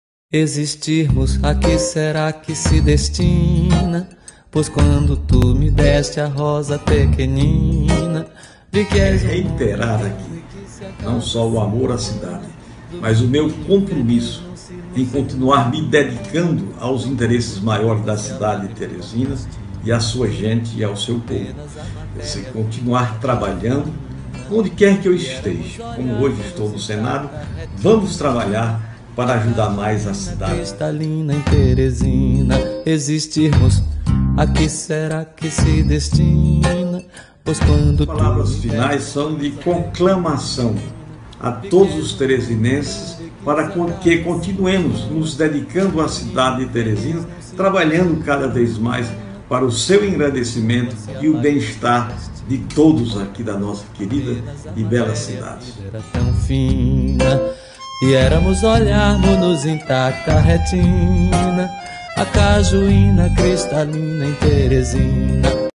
Teresina 165 anos - Homenagem do senador Elmano Férrer
16/08/2017

Homenagem do Senador Elmano Férrer na passagem do aniversário de Teresina.